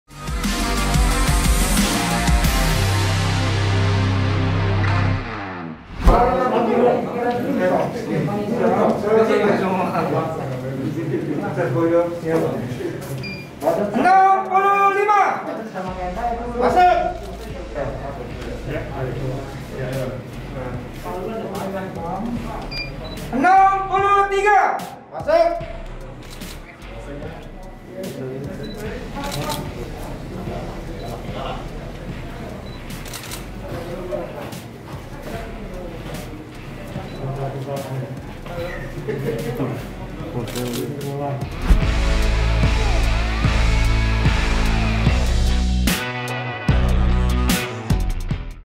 Featherweight: Ahmad Hidayat vs Toby Prasetya | Weigh In One Pride MMA FN 52
Jakarta, tvOnenews.com - Sesi Weigh In antara Ahmad Hidayat vs Toby Prasetya telah dilakukan. Saksikan pertandingan kedua petarung pada Fight Night 52 One Pride MMA - Fight for Glory, Sabtu (13/11/2021), pukul 22.00 WIB, di tvOne.